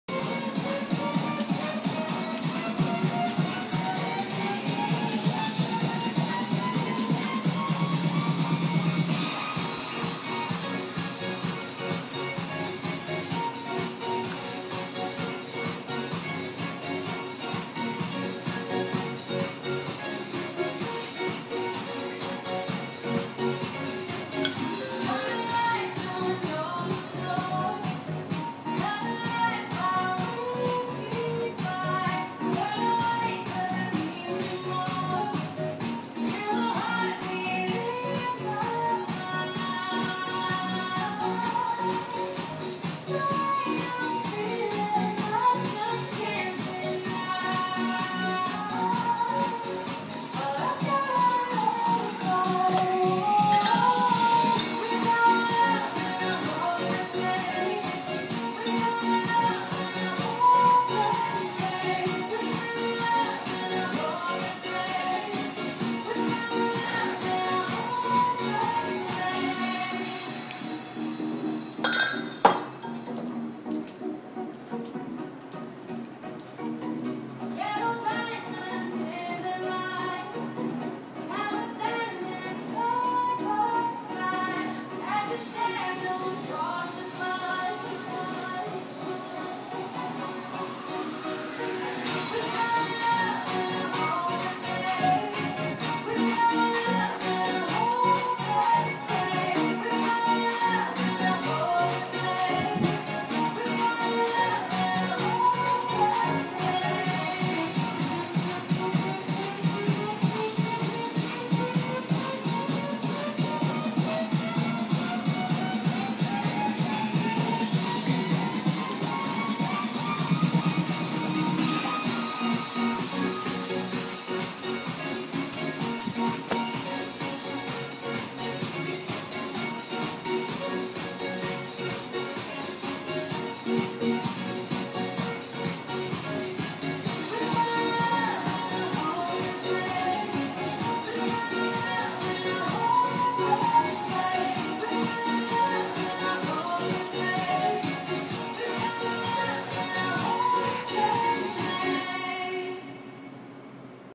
Bartender Boredumb-Part I
Just me, an empty bar, my camera phone and Pandora. Smh!